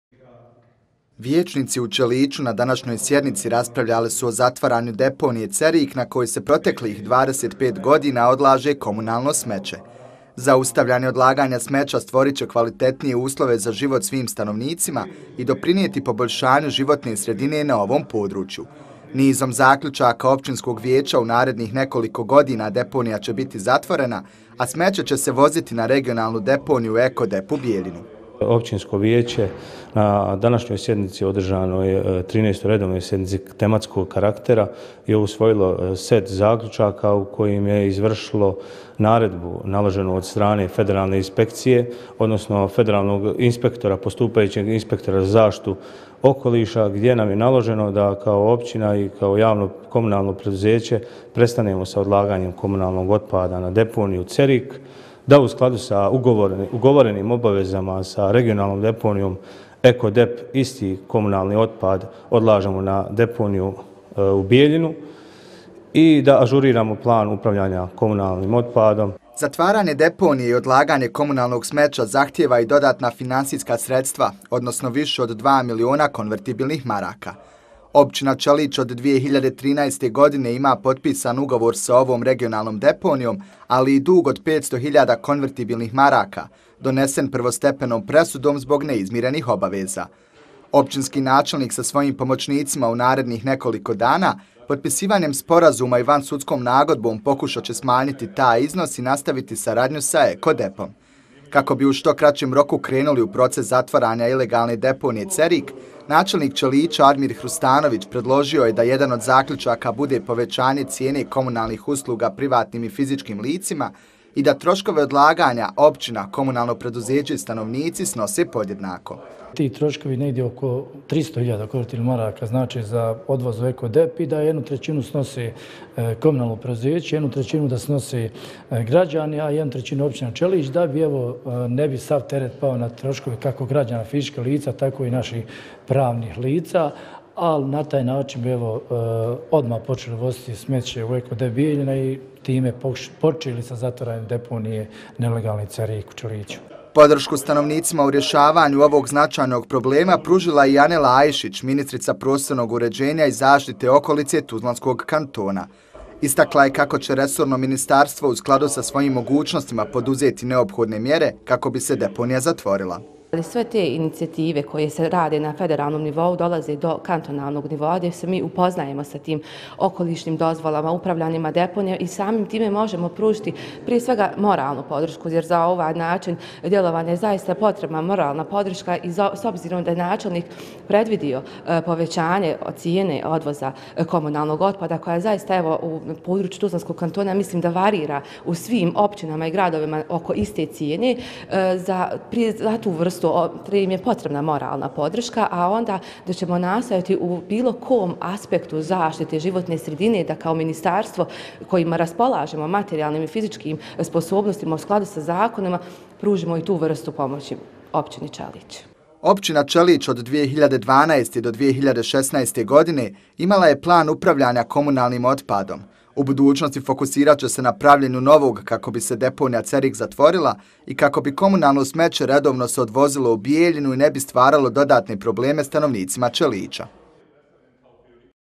Viječnici u Čeliću raspravljali o problemu odlaganja komunalnog otpada
Današnja redovna sjednica Općinskog vijeća Čelić bila je tematskog karaktera. Vijećnici su naime razmatrali rješavanje problema odlaganja komunalnog otpada i upravljanja komunalnim otpadom na području općine Čelić.  Akcenat su stavili na zatvaranje deponije Cerik I odlaganja otpada na regionalnu deponiju EKO DEP u Bijeljinu.